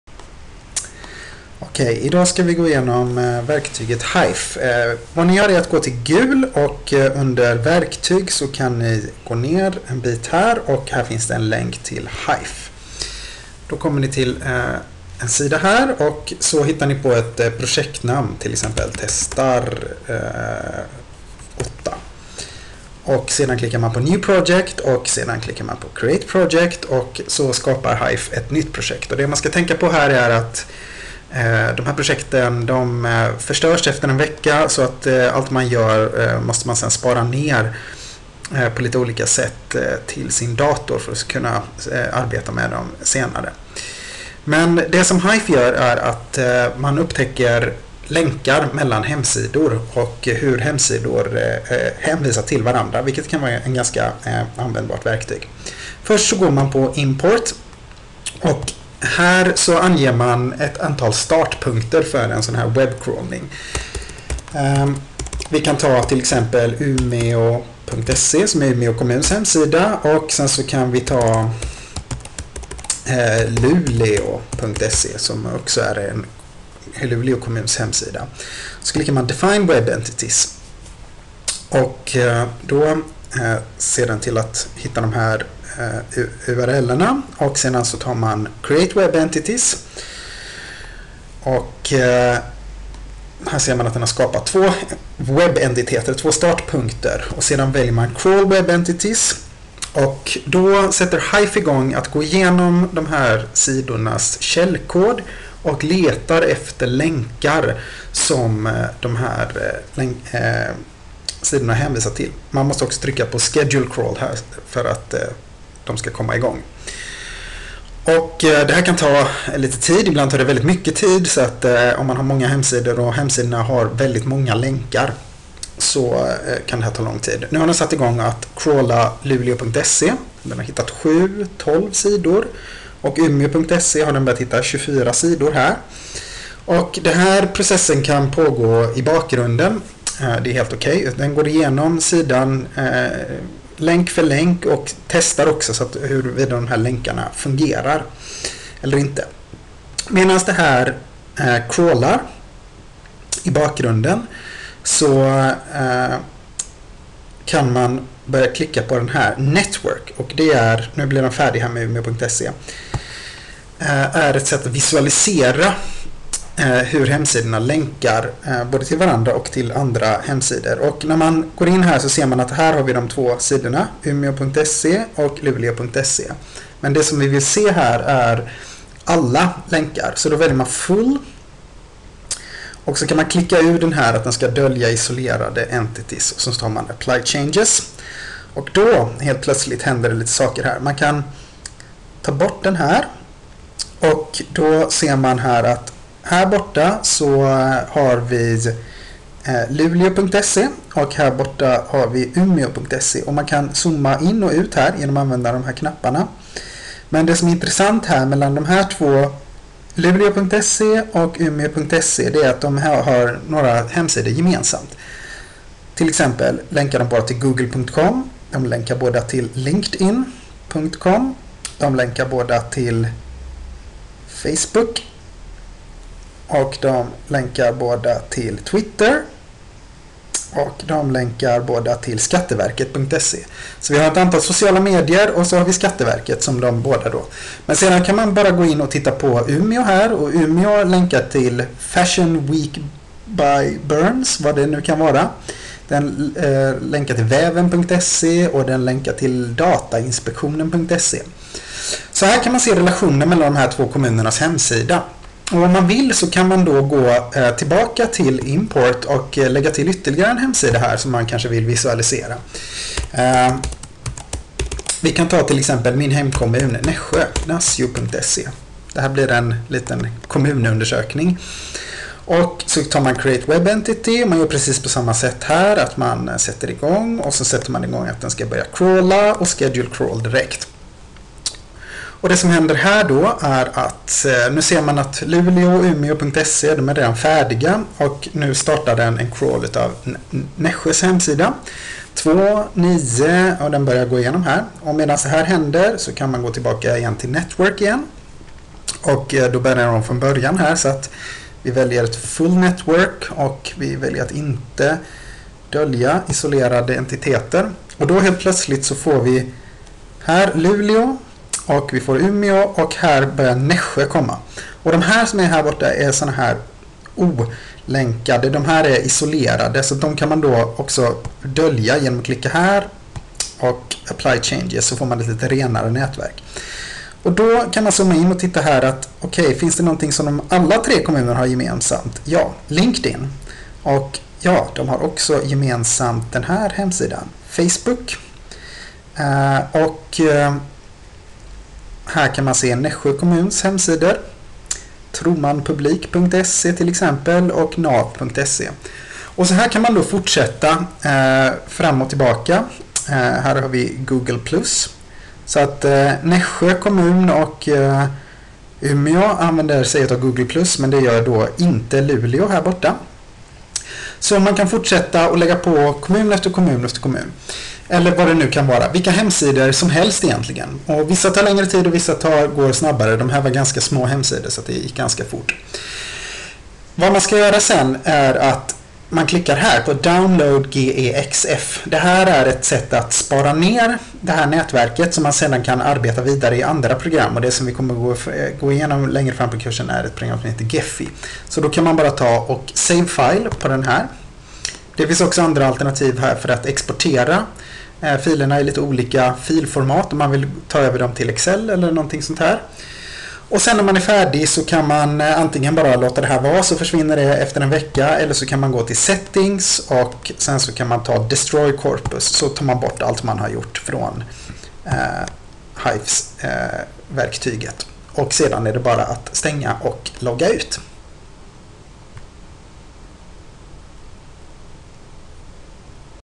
Hyphe - Instruktion på svenska
Denna video introducerar verktyget Hyphe. Används som instruktionsfilm på kursen KT2101 - Kommunikation i nya och sociala medier, Göteborgs universitet. Ljudkvaliteten är tyvärr inte den bästa.